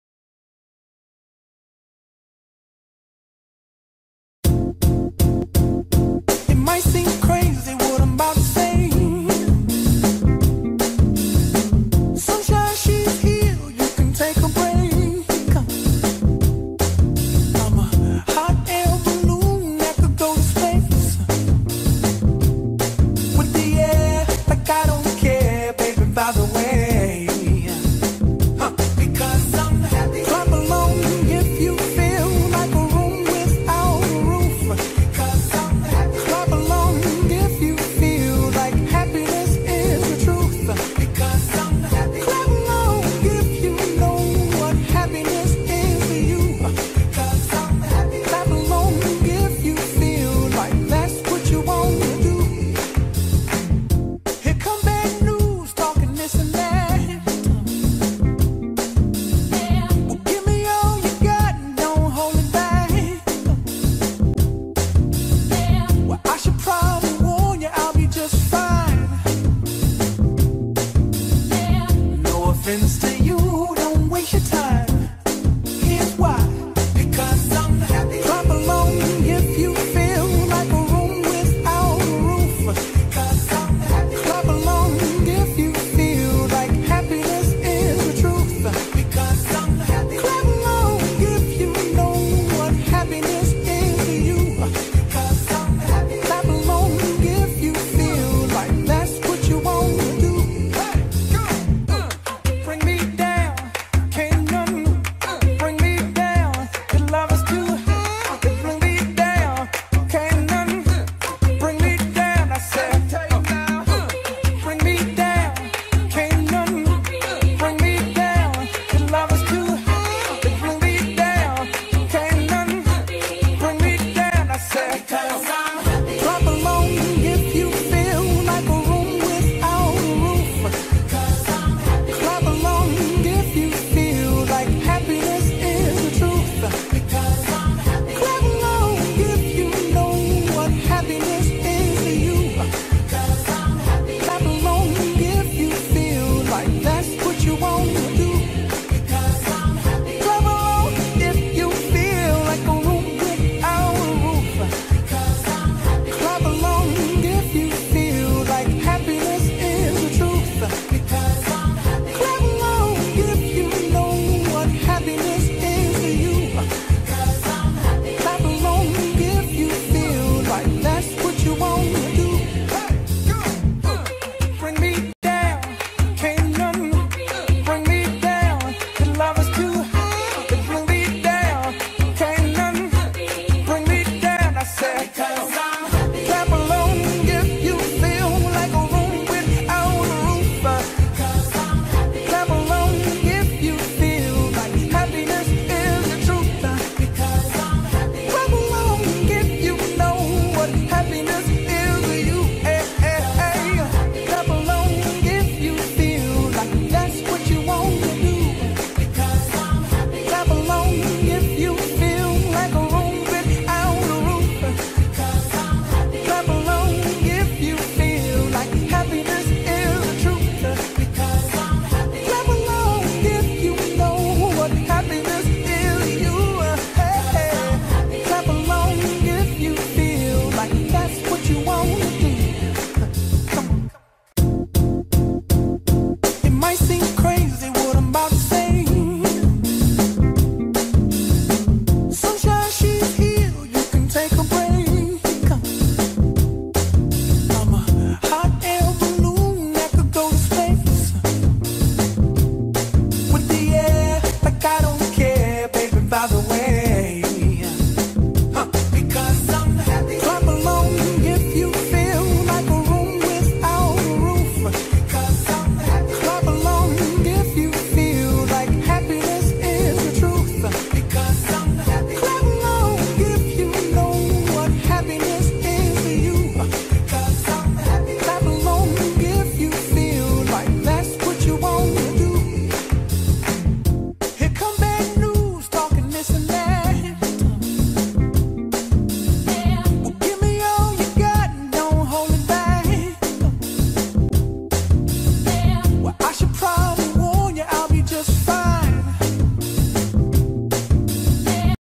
Film présentation BTSA STA 2021
Tout savoir sur la formation en BTSA Sciences et technologies des aliments (BTSA STA) dispensée par le CFA de Quetigny en partenariat avec l'IFRIA BFC